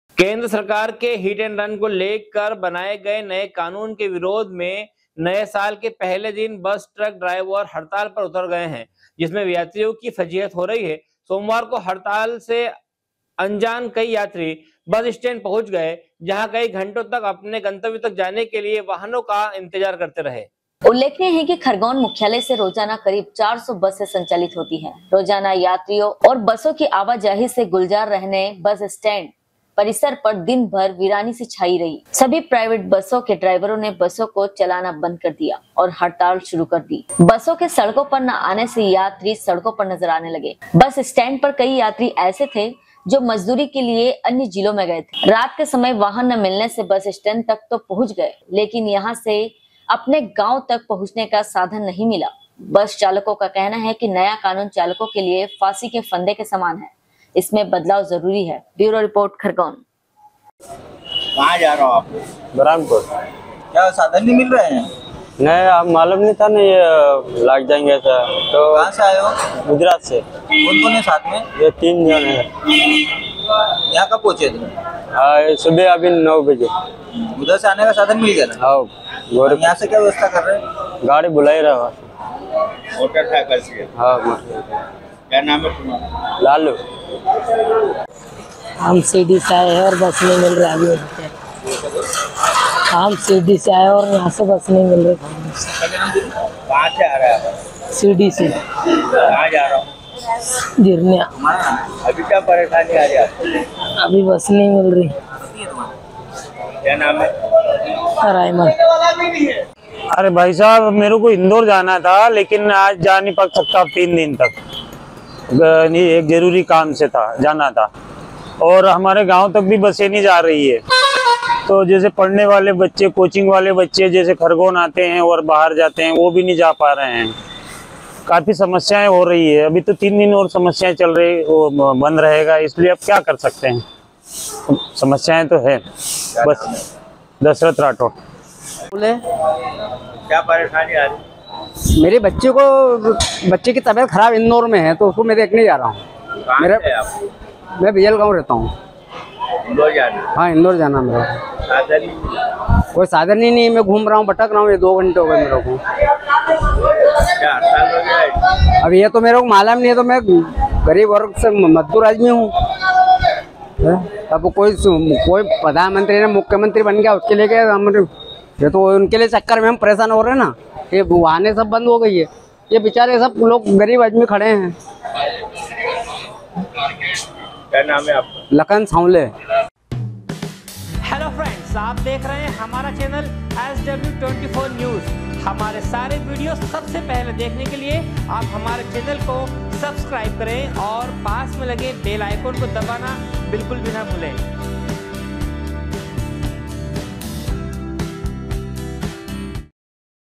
केंद्र सरकार के हिट एंड रन को लेकर बनाए गए नए कानून के विरोध में नए साल के पहले ही दिन बस,
केंद्र सरकार के हिट एंड रन को लेकर बनाए गए नए कानून के विरोध में नए साल के पहले ही दिन बस, ट्रक ड्रायवर हड़ताल पर चले गए है, जिससे यात्रियों की फजीहत हो रही है। सोमवार को हड़ताल से अनजान कई यात्री बस स्टैंड पहुंच गए जहां कई घण्टो अपने गंतव्य तक जाने के लिये वाहनों का इंतजार करते रहे।